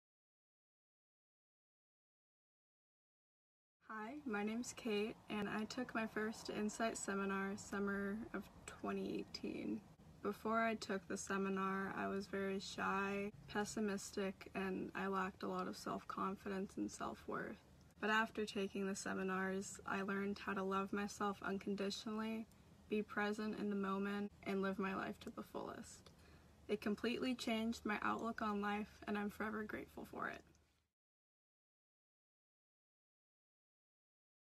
Grad Stories: Meet Teen Insight Grad, Kate
Meet Teen Insight Grad, Kate! She most recently graduated Teen Insight III during the summer of 2019. We can’t wait for the upcoming Teen Insight I Dec. 27-30!